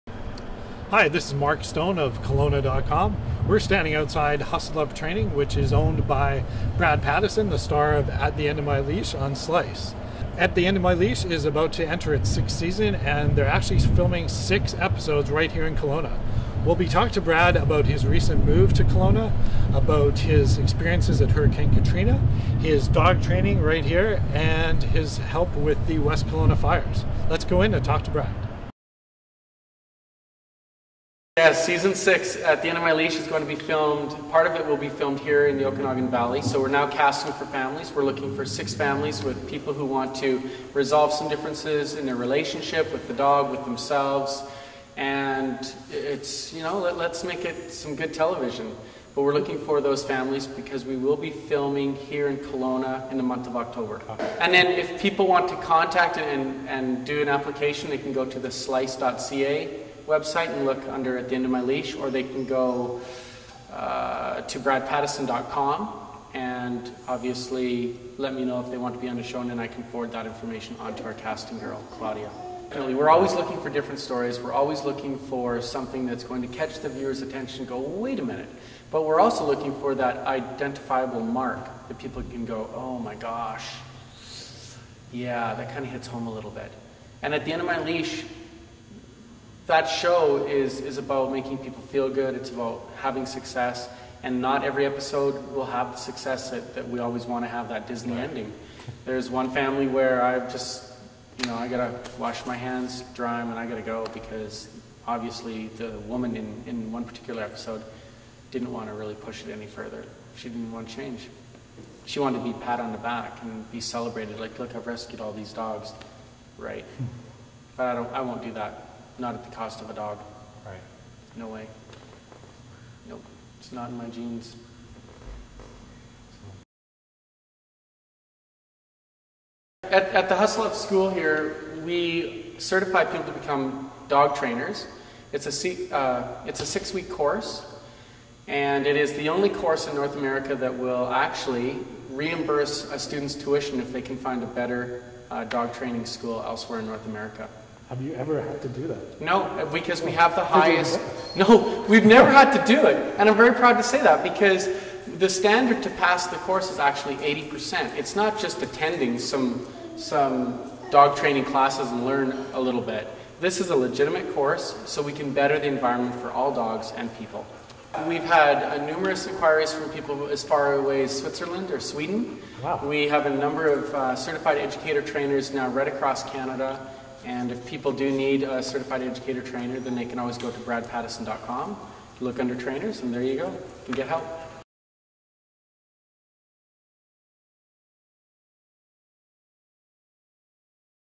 Brad Pattison talks to Mark Stone of Kelowna.com
Brad Pattison, canine behaviourist on the television show At The End Of My Leash talks to Kelowna.com.